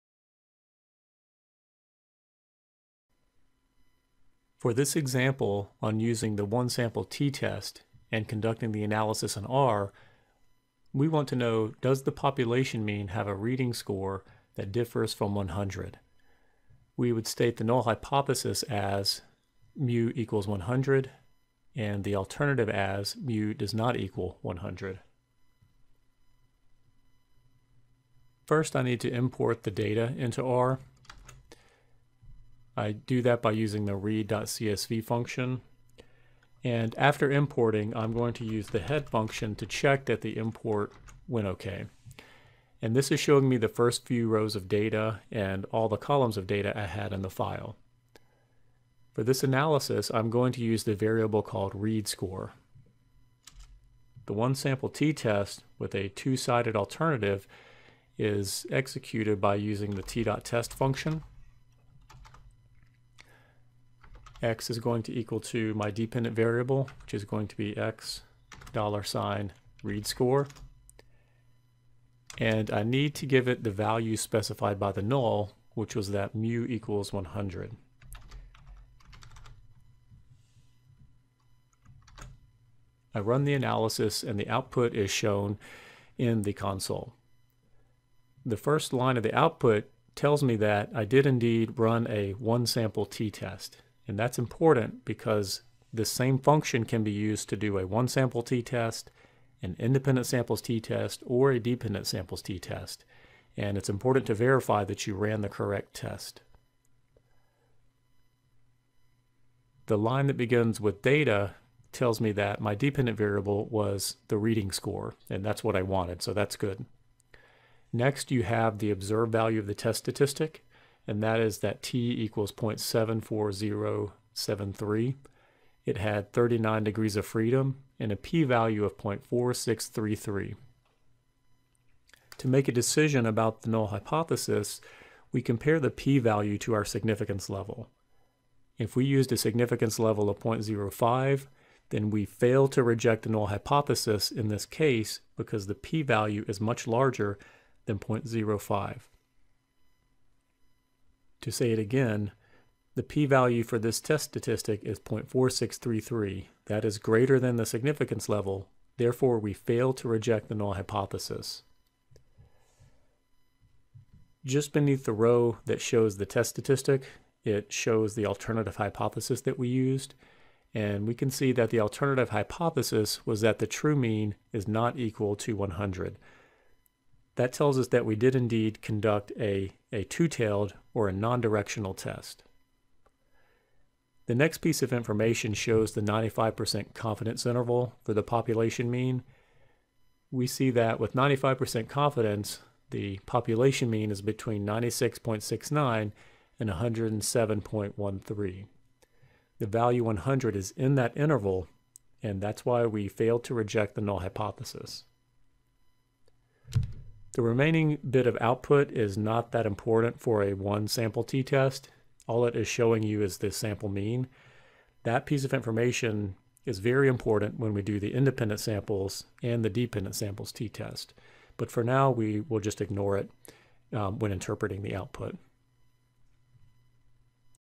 Using R for a One-sample t-test
Explains how to use R for the one-sample t-test. It includes an explanation of the function and interpretation of the output.